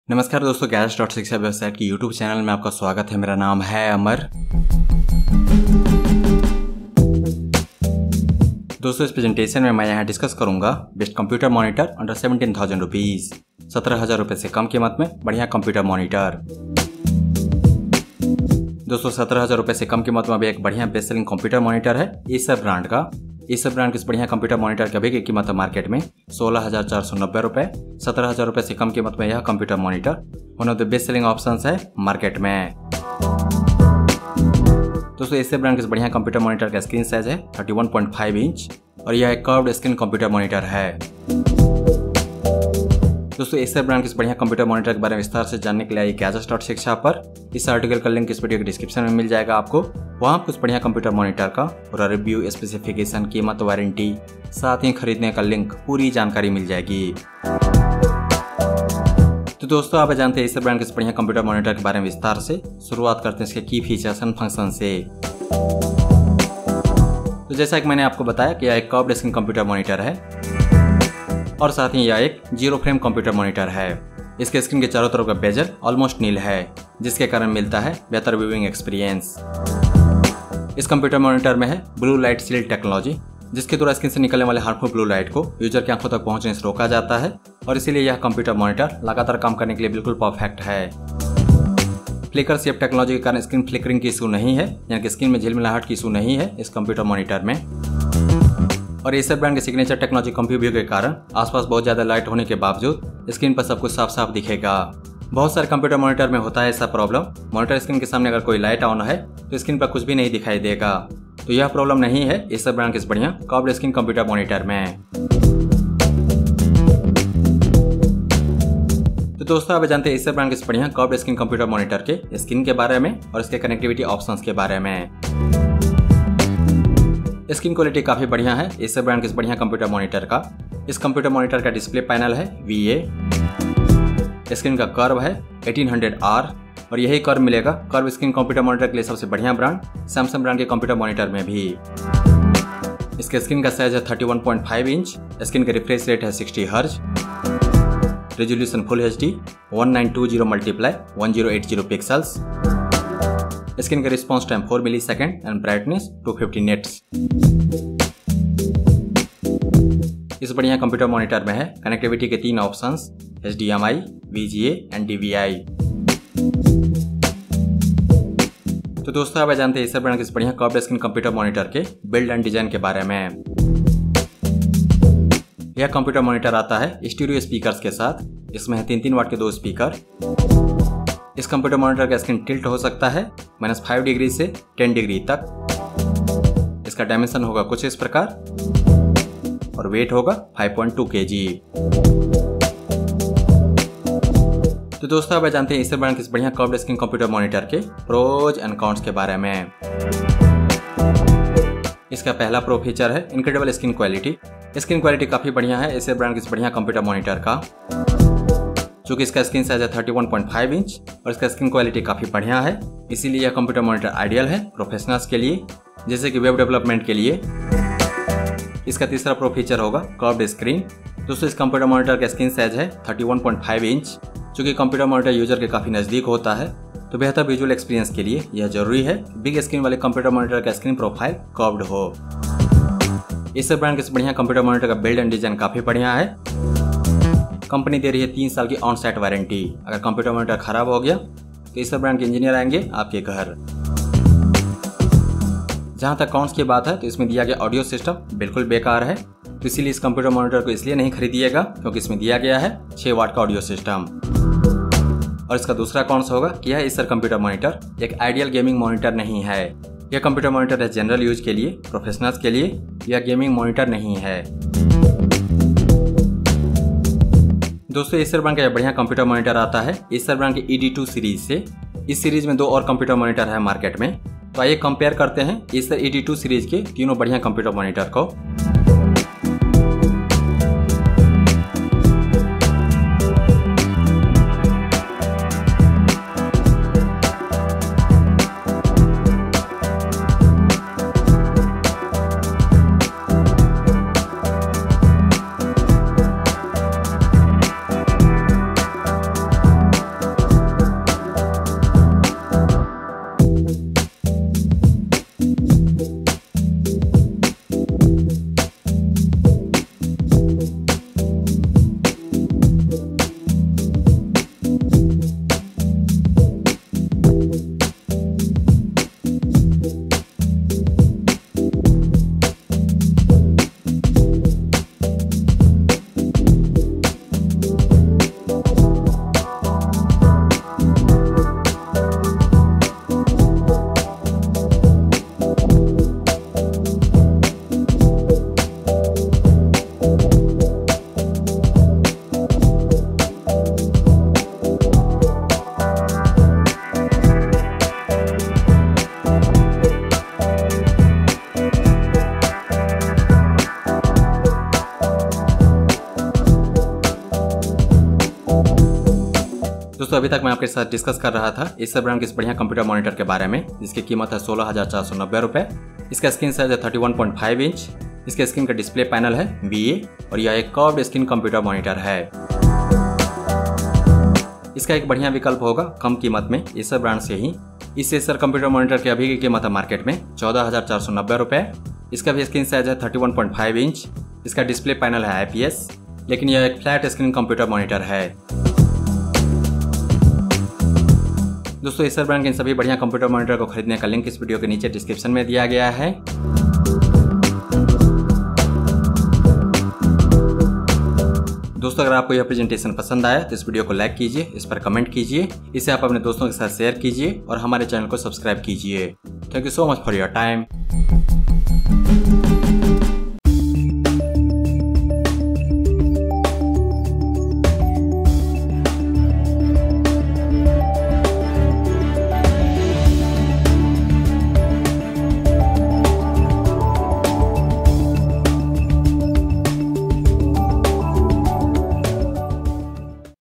🔥31.5-Inch ► Curved Computer Monitor under 17000 {हिंदी में} ► Acer ED322Q ► Acer Curved Monitor
The Acer ED322Q curved monitor is one of the best computer monitors in India market under 20000 Rupees. Its screen size is 31.5-Inch and screen panel VA. With all its qualities, features, and functions this Acer full HD computer monitor is an excellent choice for professionals as well besides being the most favorite option for general use. This presentation covers all the details of the Acer Curved Screen Monitor.